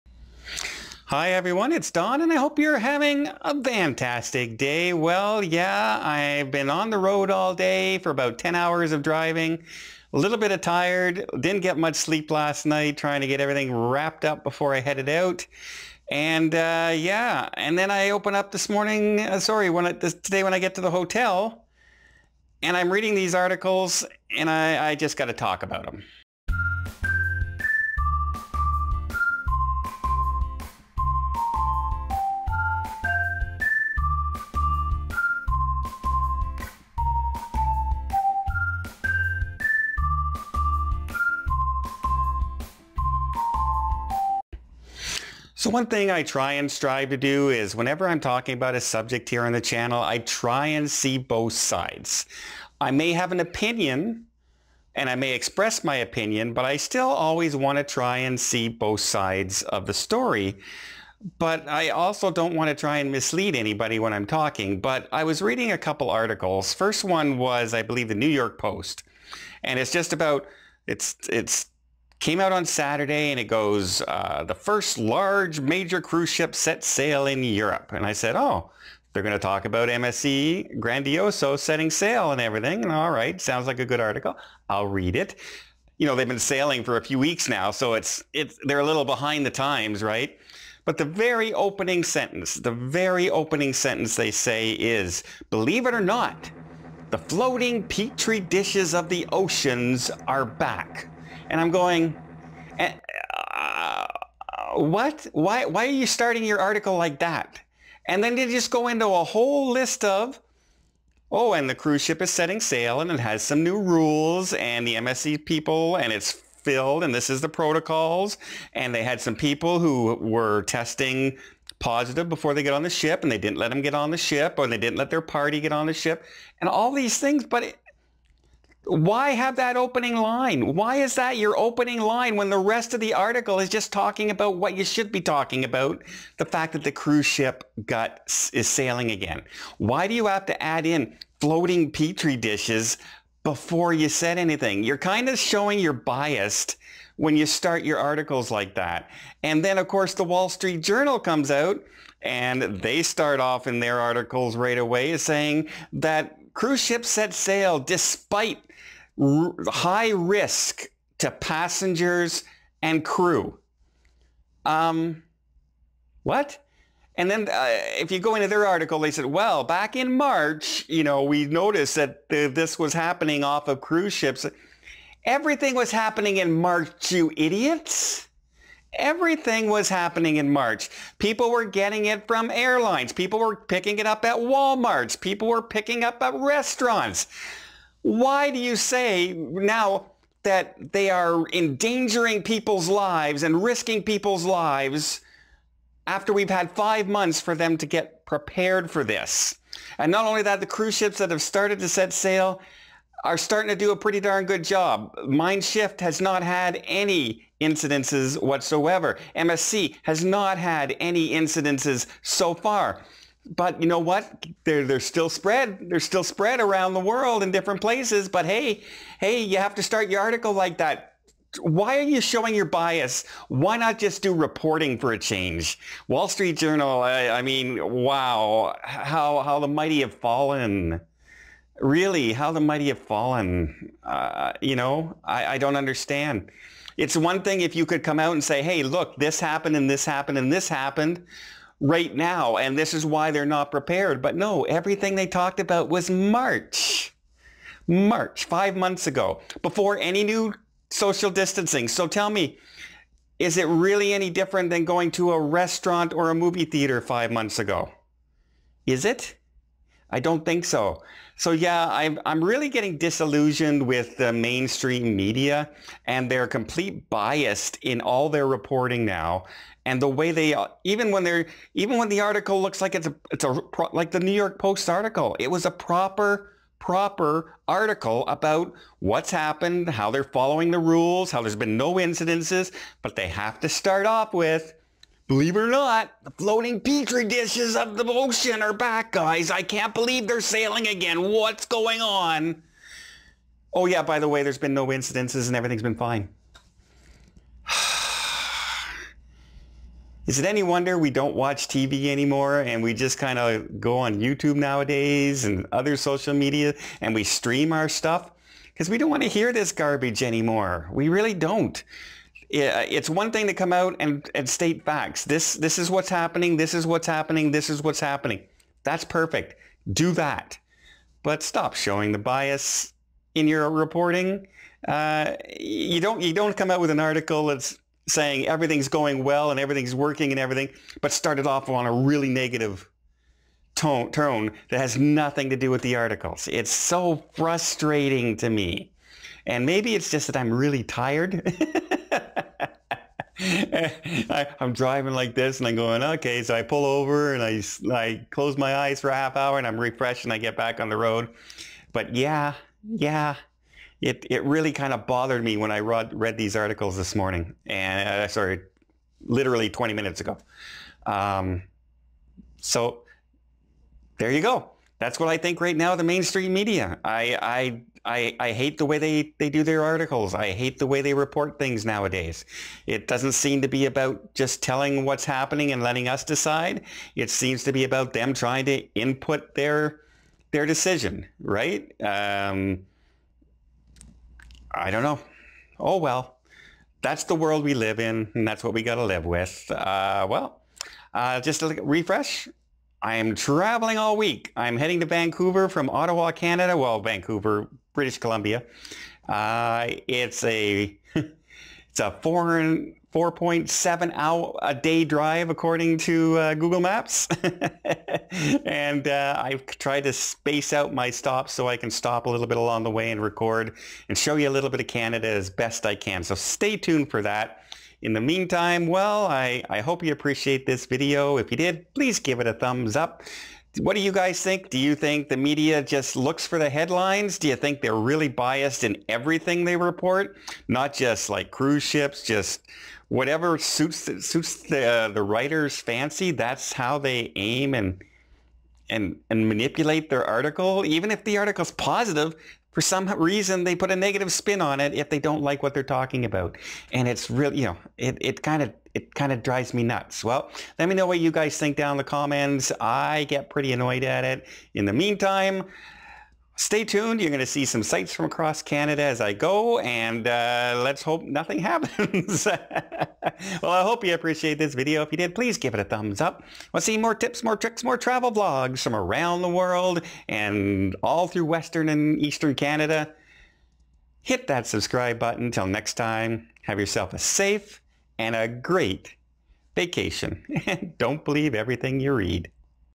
Dear Media Your Bias Is Showing - Cruise Ship News
Dear Media Your Bias Is Showing. I notice a trend in the way the media reports on Cruises. Even when they are reporting things going well and positive news. They have to add some comment or section to put a negative spin on the industry. I just don't understand why they can't just report the news the way it is happening in the cruise ship industry?

For those of you who are wondering, yes I am a Travel Agent and I work with anyone in Canada and the United States :)